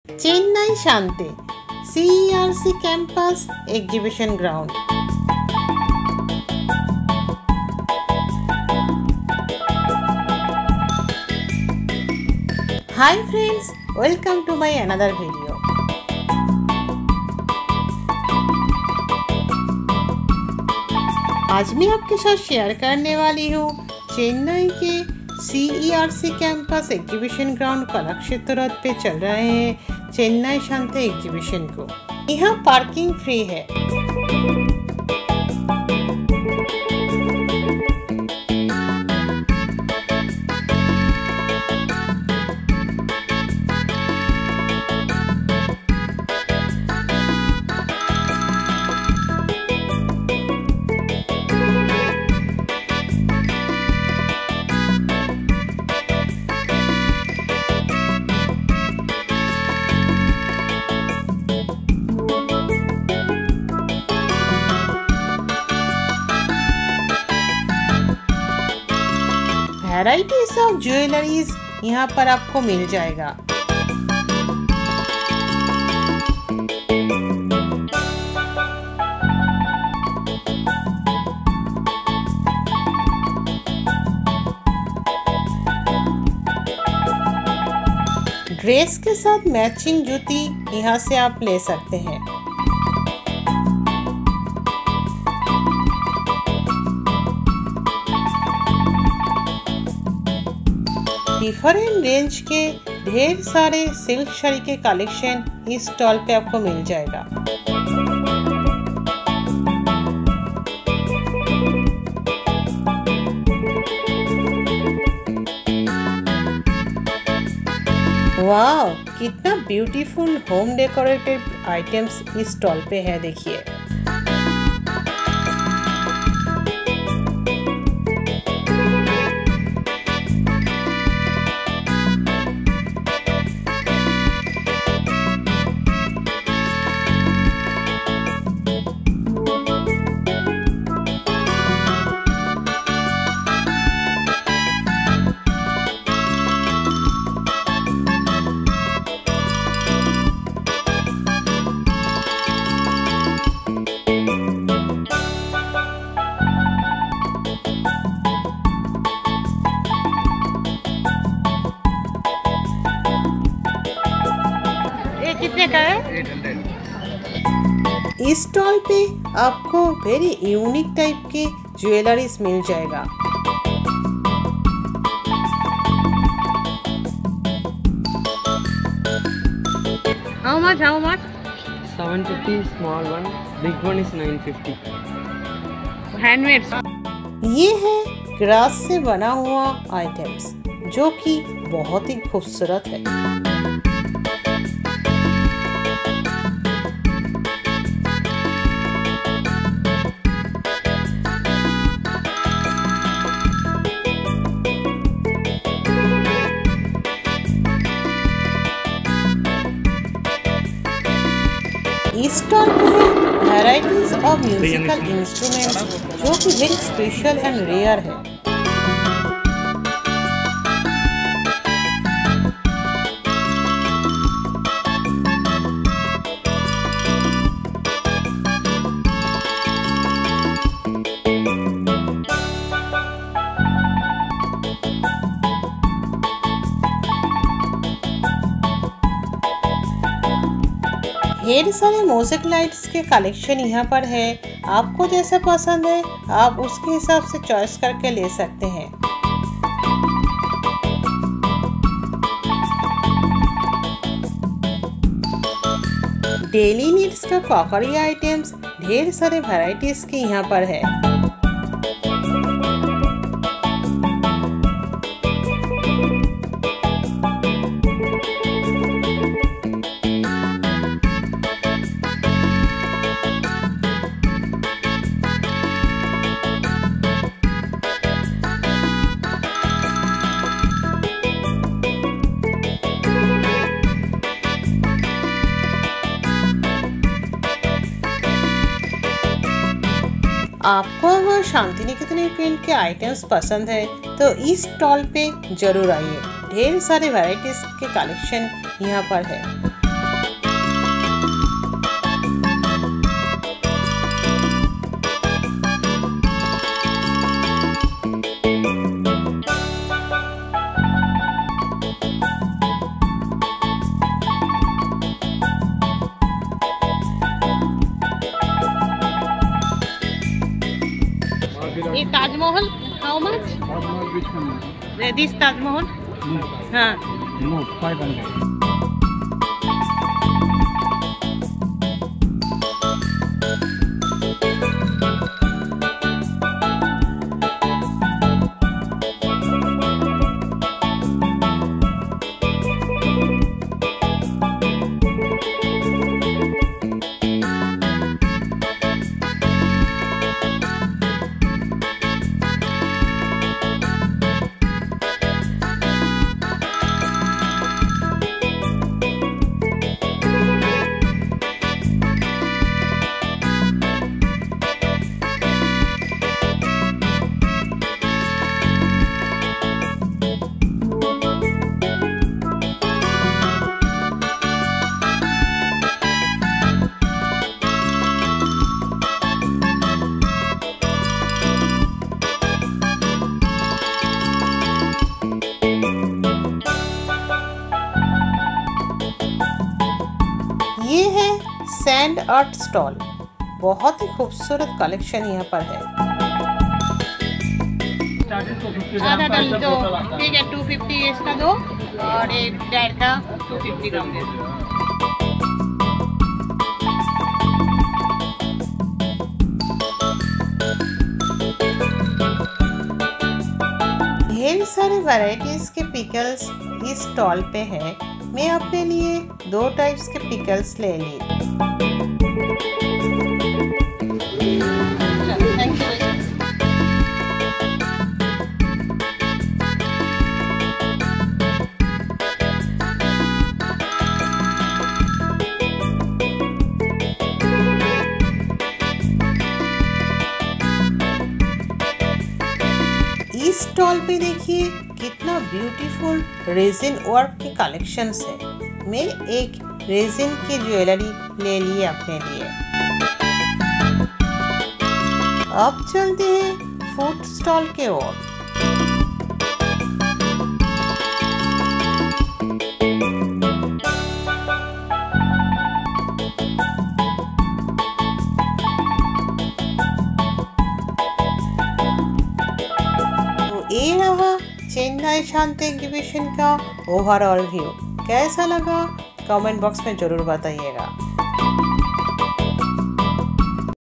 CHENNAI SANTE Exhibition 2024 at CERC Campus Exhibition Ground
CHENNAI SANTE Exhibition 2024
CERC Campus Exhibition Ground
Kalakshetra Road,
Thiruvanmiyur, Chennai - 600041

CRAFTS  ||  TEXTILES  ||  HOME DECOR  ||  JEWELLERY

24 May 2024 to 02 June 2024
11:00 AM to 9:00 PM

Entry & Parking Free